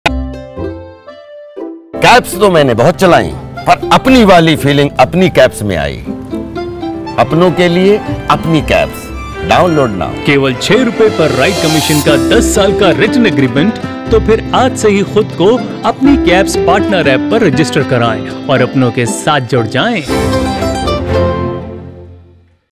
Apni Cabs के साथ अब हर Ride पर कमाई होगी ज्यादा | ₹6 फिक्स्ड प्लेटफार्म फीस हर राइड पर...
Rajpal Yadav के भरोसे के साथ, अब ApniCabs के साथ अब हर Ride पर कमाई होगी ज्यादा. ₹6 फिक्स्ड प्लेटफार्म फीस हर राइड पर...